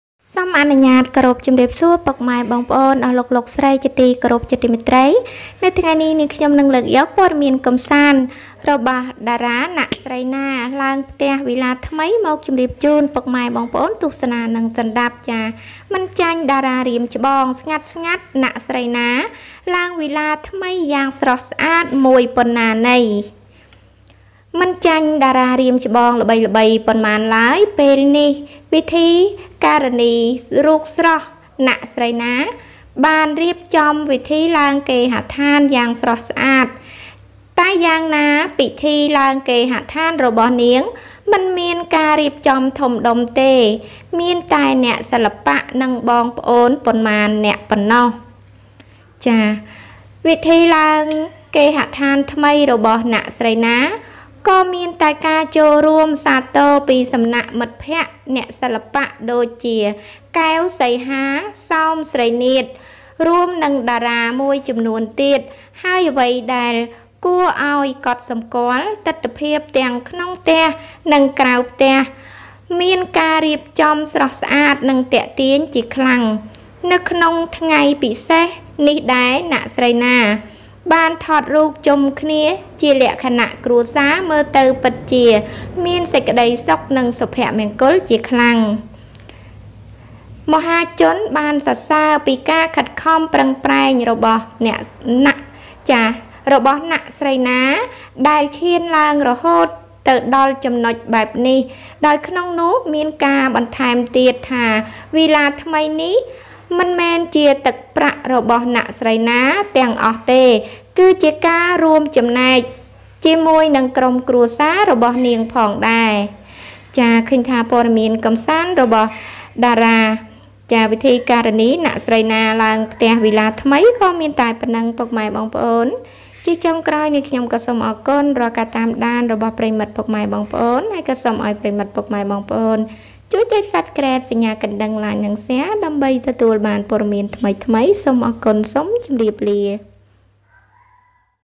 មិនចាញ់តារារៀមច្បង ស្ងាត់ៗ ណាក់ ស្រីណា ឡើងវីឡាថ្មីយ៉ាងស្រស់ស្អាតមួយធំប៉ុនណាណី
Silent starlet Nan Sna Na climb a beautiful new villa

channel/KhmerNewsToday